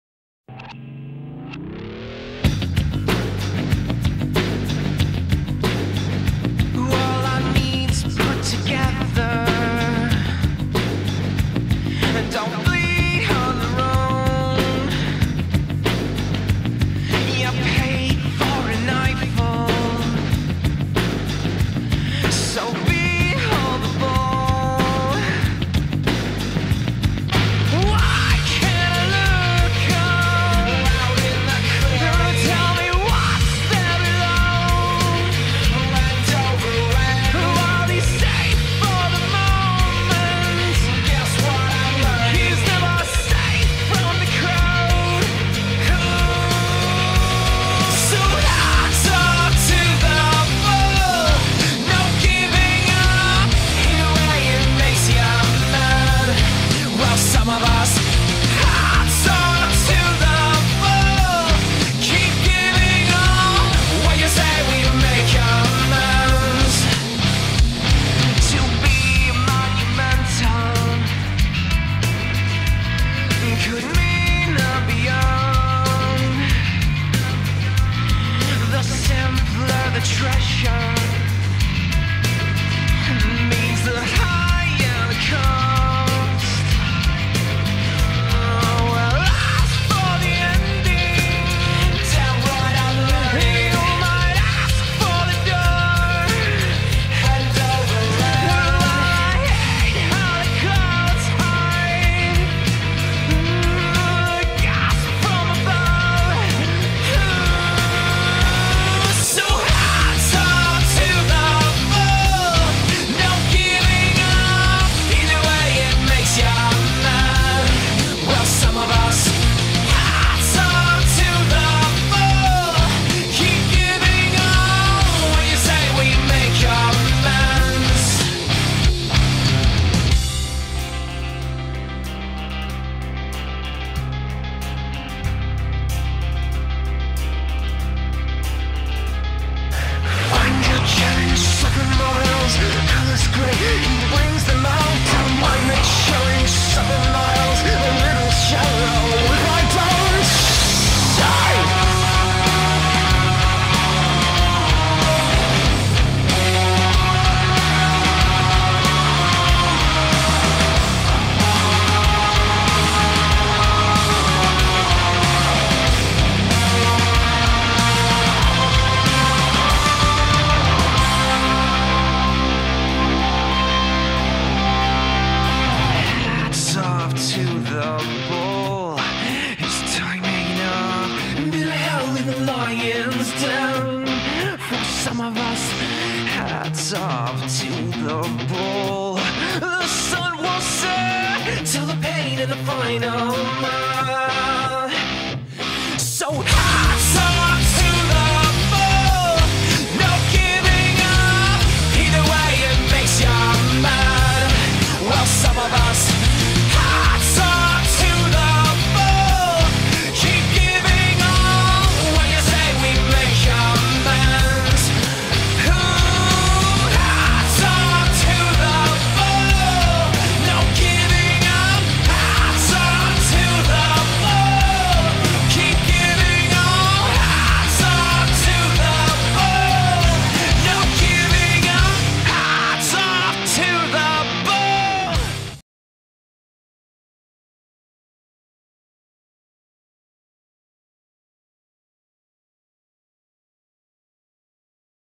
Spanish Forces/Special Forces|2016|~No Giving Up~
Tribute to brother's and sister's in Spanish Armed Forces, and Special Opertations thank you guys for your service.Also, any request or feedback is fine.Hoped you guys enjoyed the video!